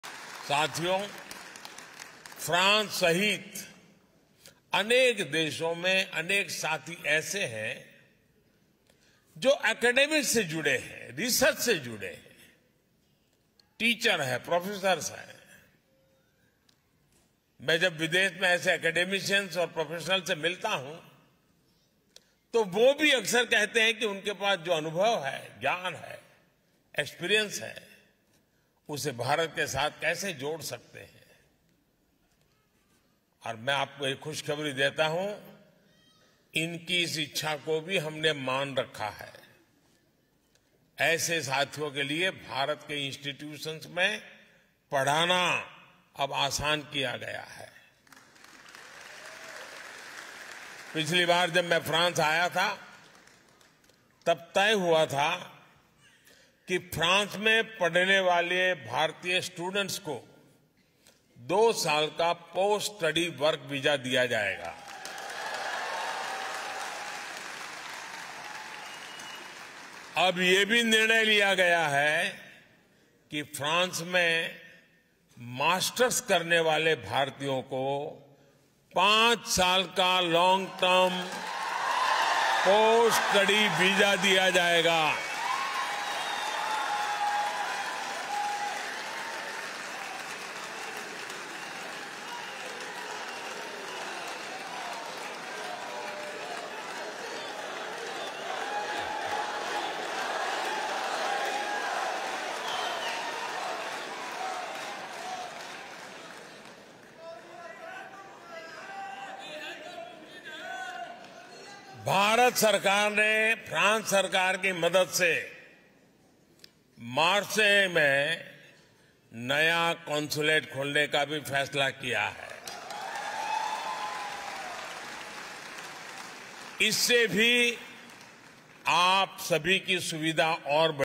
Indians studying in France will be given long term post study visa of 5 years, PM Modi announces
PM Modi interacted with the Indian diaspora in France. He highlighted the multi-faceted linkages between India and France. He  appreciated the role of Indian community in bolstering the ties between both the countries.

The PM also mentioned the strides being made by India in different domains and invited the diaspora members to explore opportunities of investing in India.

PM Modi in Paris

Modi in Paris

Modi in France

France

PM Modi in France

Paris

Indian Community